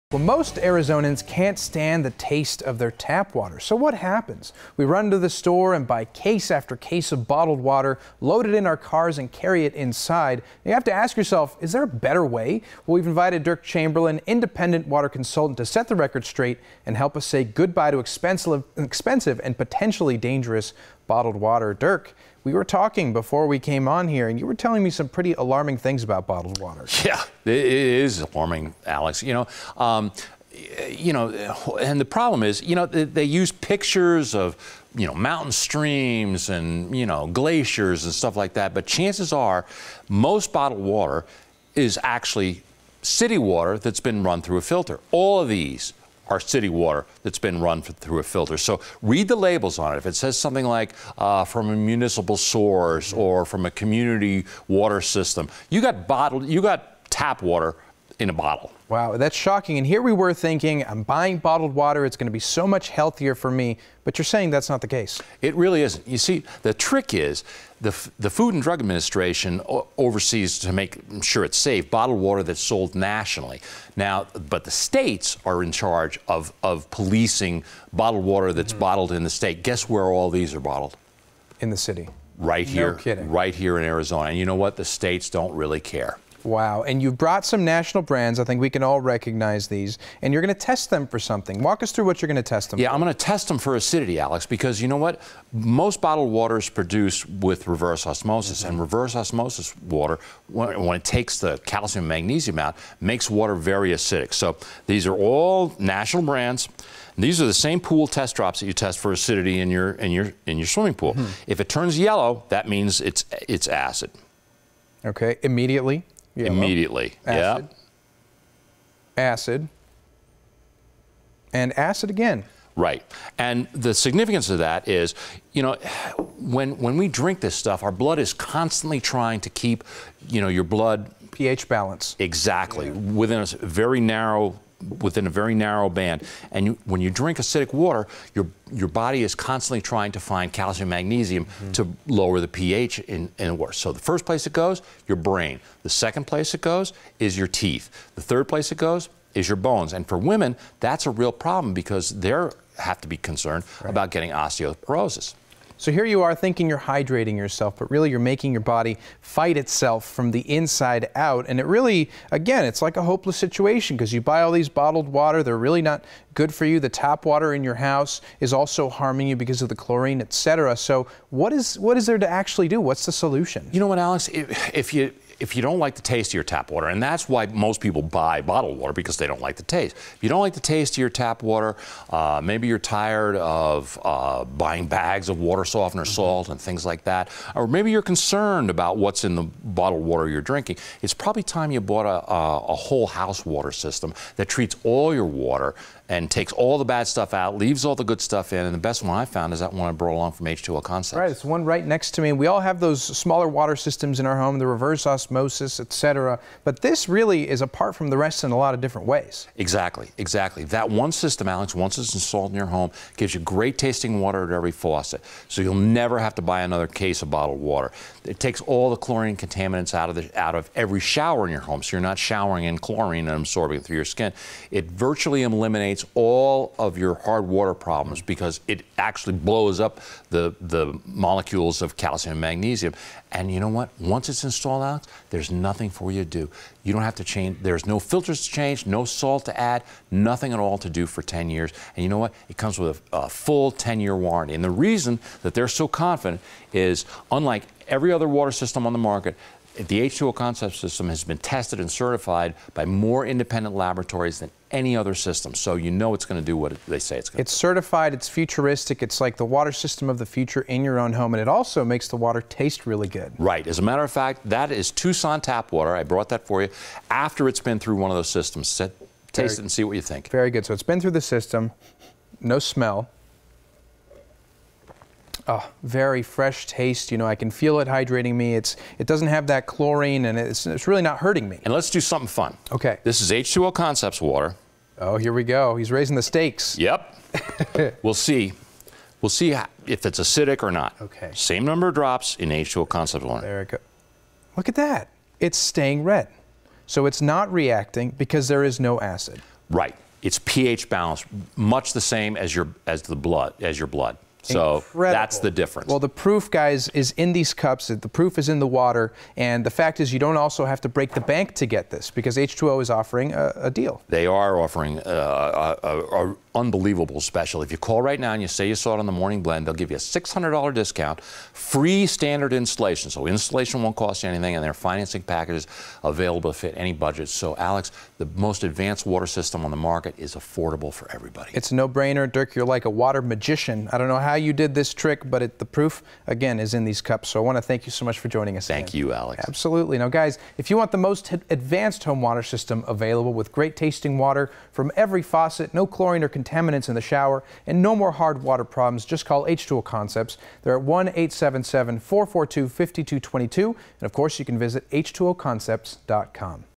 Is bottled water ACTUALLY better than tap water?
Those pictures of glaciers or pristine mountain streams on bottled water may be a lie